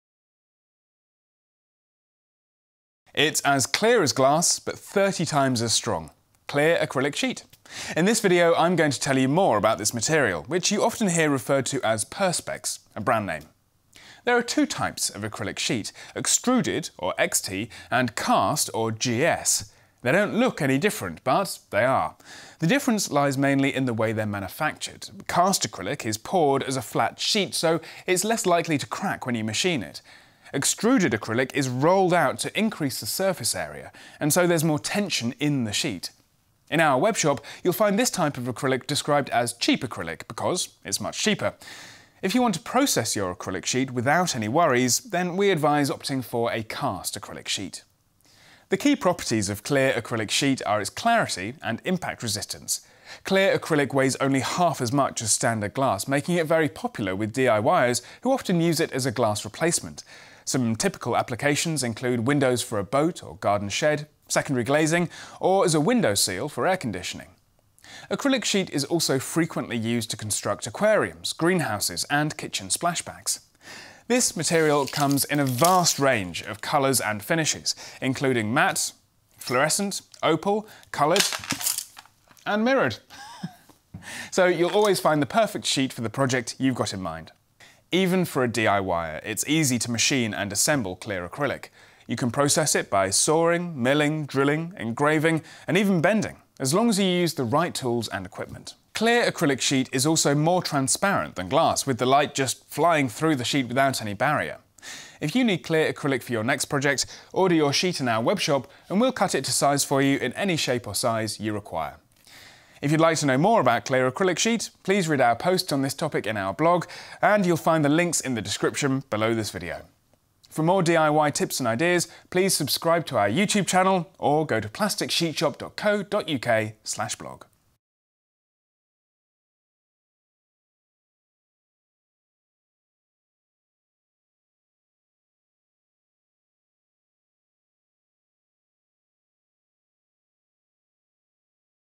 Acrylic Sheets: Everything you need to know | Plasticsheetsshop.co.uk
It’s as clear as glass, but thirty times stronger: clear acrylic sheets. Plasticsheetsshop.co.uk offers high quality clear, opal, tinted, frosted and coloured acrylic sheets! In this video, we’ll tell you more about this material, which is also known as perspex®️ or acrylic glass.

Advantages of acrylic sheet:
✓ Twice as light and 30 times as strong as traditional glass
✓ Just as clear as glass
✓ Much cheaper than glass

Would you like to order custom-cut acrylic sheet in any shape?

Would you like to know more about acrylic sheet? Read our blog about acrylic sheet